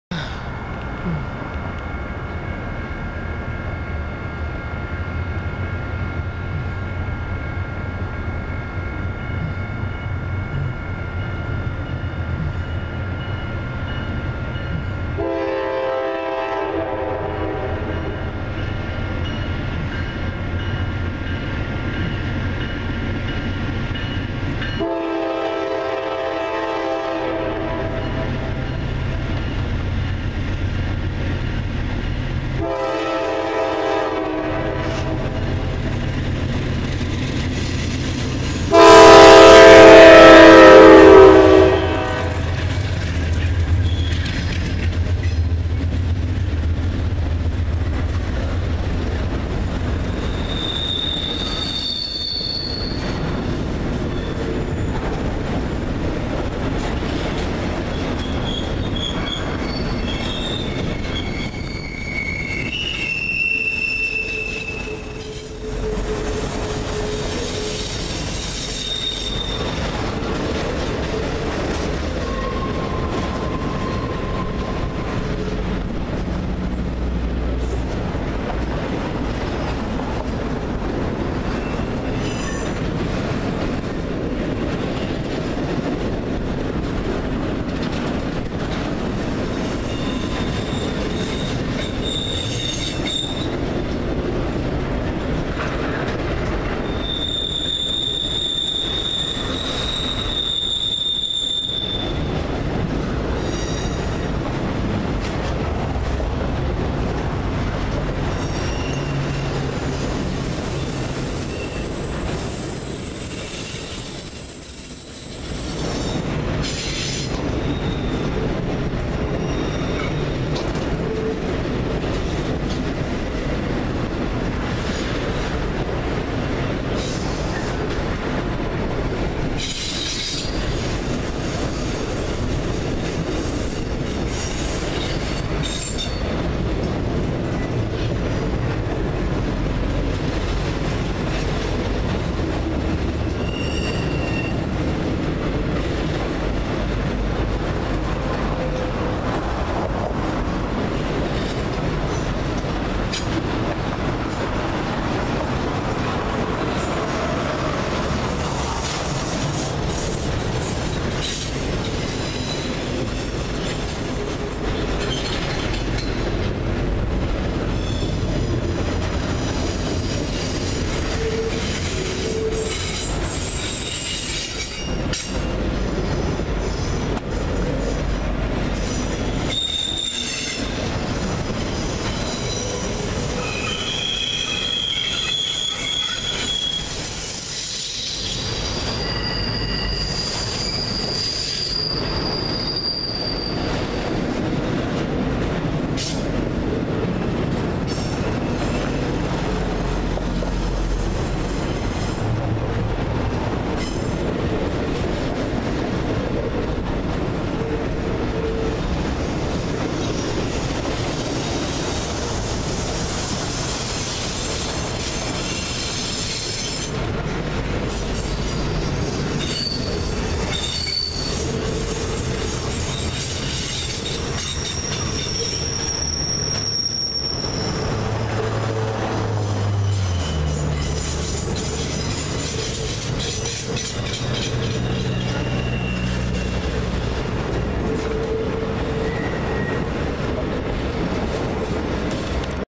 Norfolk Southern Intermodal Train in Harrisburg, PA #2 - 9/4/16
A Norfolk Southern intermodal train is seen crossing Post Office Road on the Harrisburg Line with a Nathan K5LA horn in Harrisburg, Pennsylvania at 1:05 PM on Sunday, September 4, 2016. The engines in this video are: NS 9667 (D9-44CW), NS 9490 (D9-44CW) and NS 9238 (D9-44CW). Enjoy!